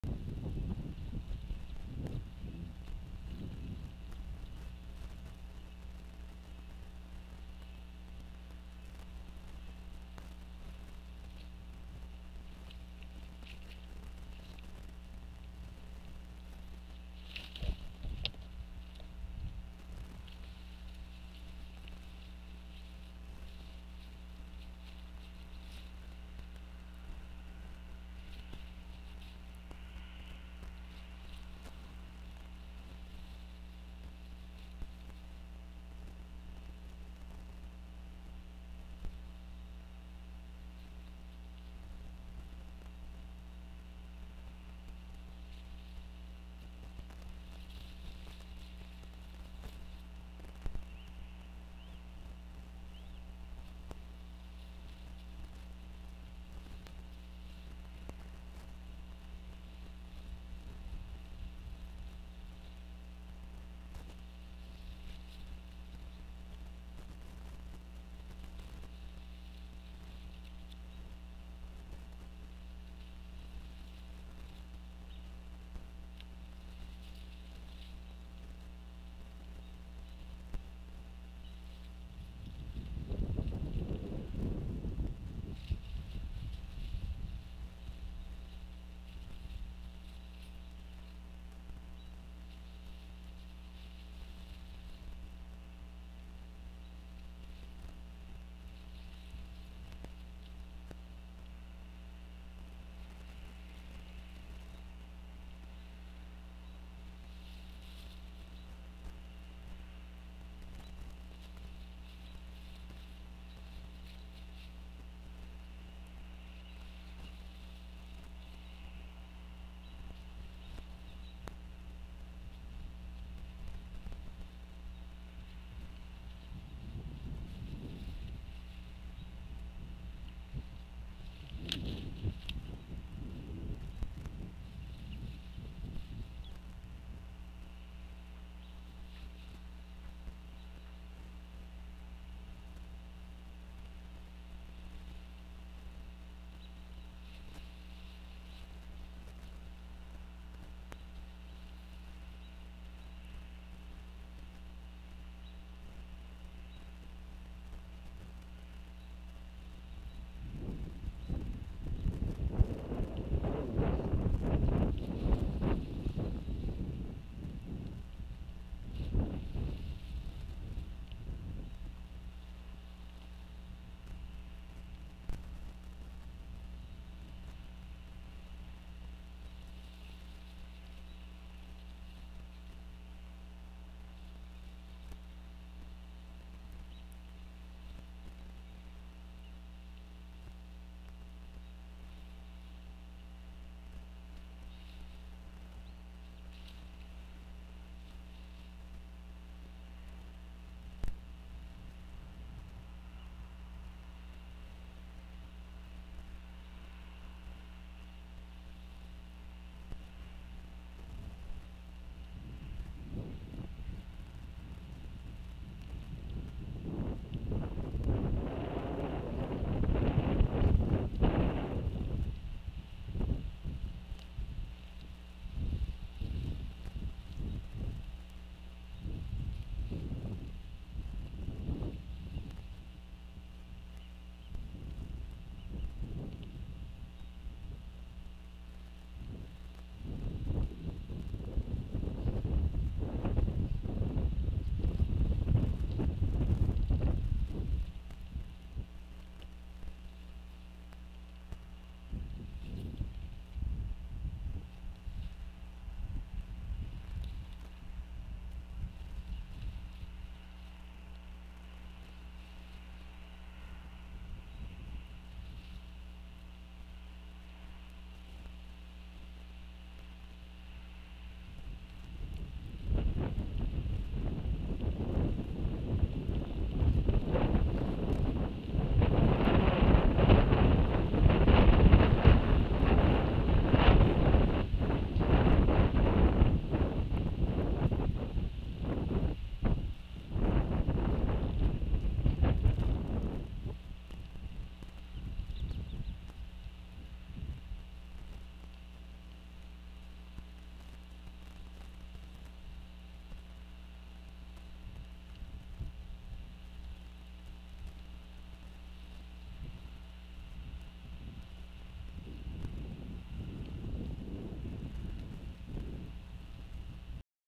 AEF 12-24-2013 Northeast Florida Bald Eagles-NEW BLANKET
Juliet Fly's in with some big nest material and it is a new Blanket for the Lil ones.!!